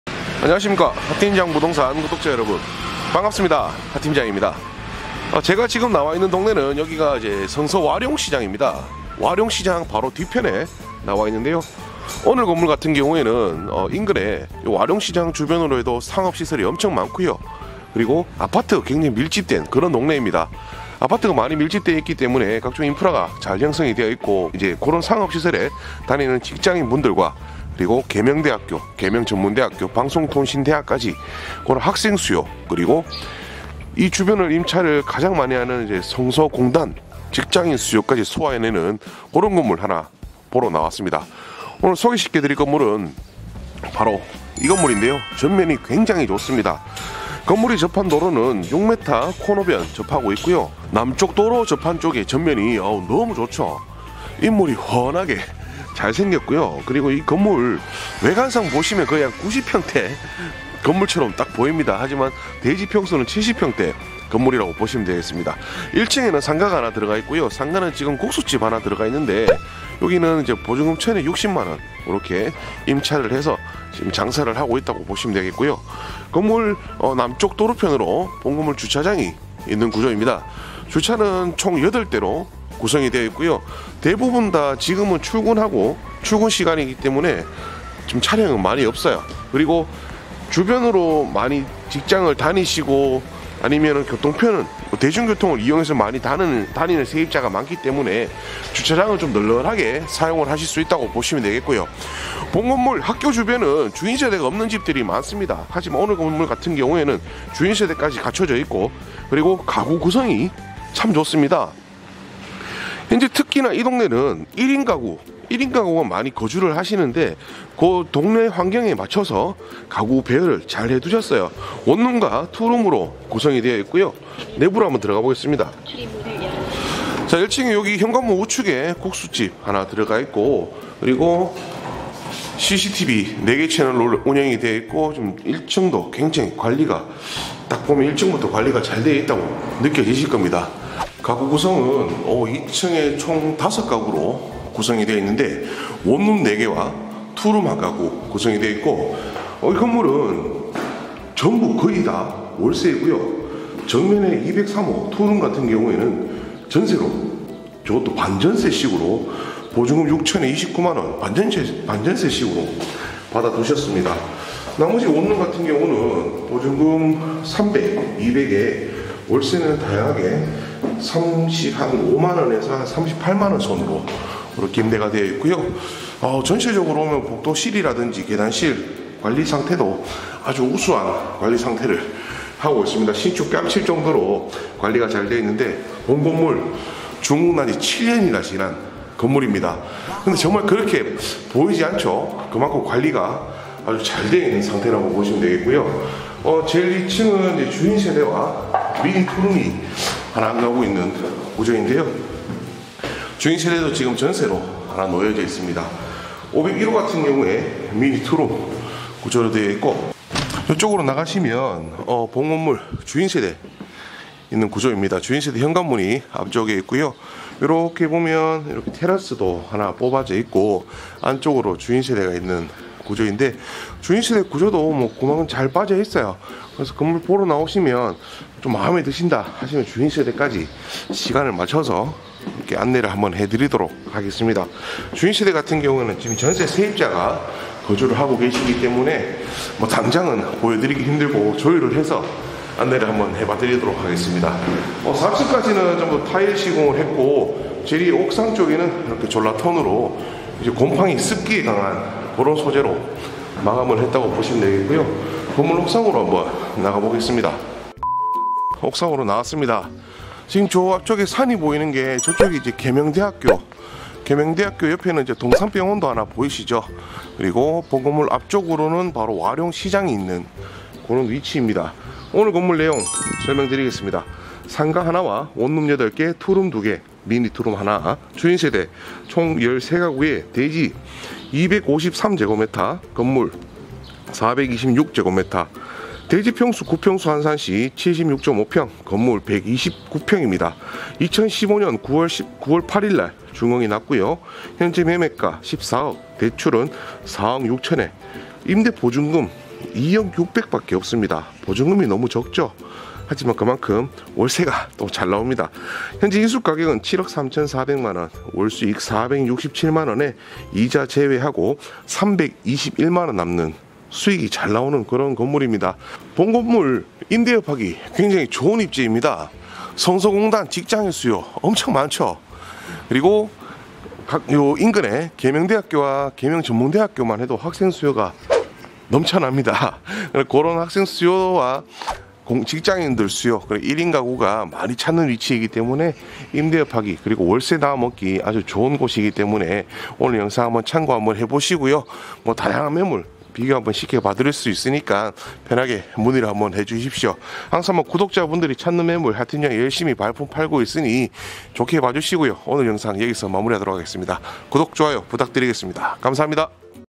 [하팀장]대구원룸매매 거품 쫙~빠지고도 수익 잘~나오고 인프라 완벽 구축 된 위치에 고수익 코너건물 소개해봐드릴게요^^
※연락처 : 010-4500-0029
※카카오톡 ID : 45000029 (검색)
※해당영상 매물번호:040722

▶소재지 : 대구광역시 신당동 170
▷면적 : 대지 253㎡ / 연면적 426㎡

▶거래형태 : 매매
▷거래예정금액 :14억원
▶중개대상물의 종류: 다가구주택

▷사용승인일 : 2015년09월08일
▶총 층수 : 지하 0층 / 지상 4층
▷주차대수 : 8대

▶방향[주 출입구 기준] :동
▷입주가능일 : 2022년04월11일이후 협의가능
▶방수[근린생활시설 미포함] / 욕실수 : 방 15개 / 욕실 13개

▷관리비[청소비용,승강기 유지비용 등] : 매월 약 200,000원 예상

◆대표:沈民貞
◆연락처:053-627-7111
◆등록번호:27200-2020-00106